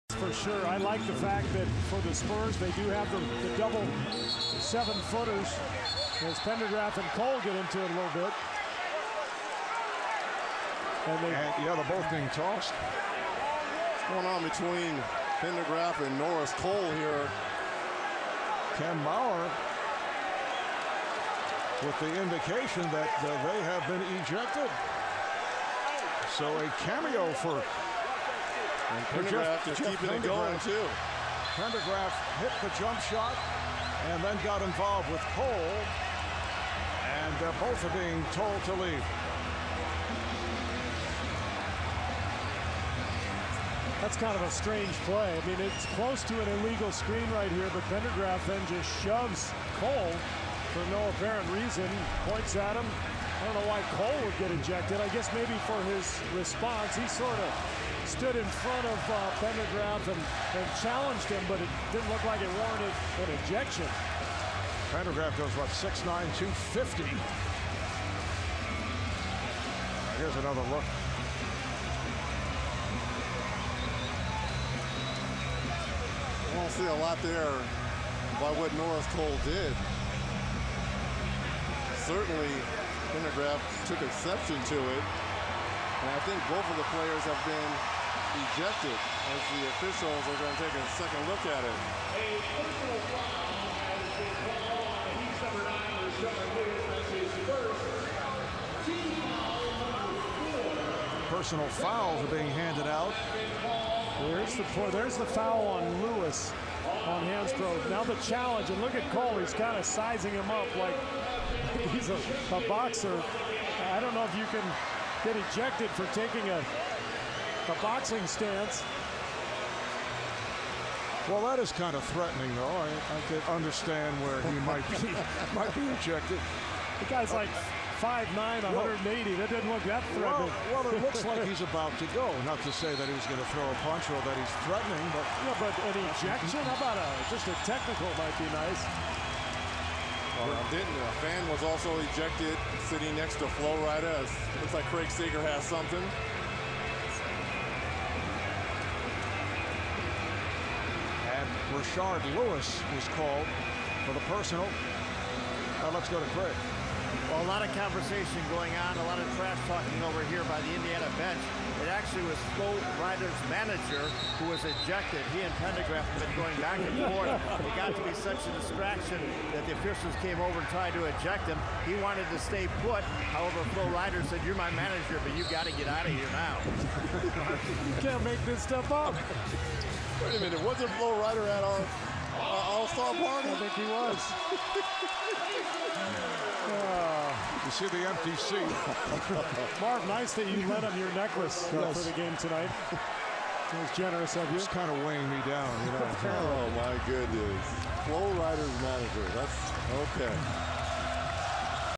Strong Arm Live At Game 7
Strong Arm Team getting live at the Miami Heat Game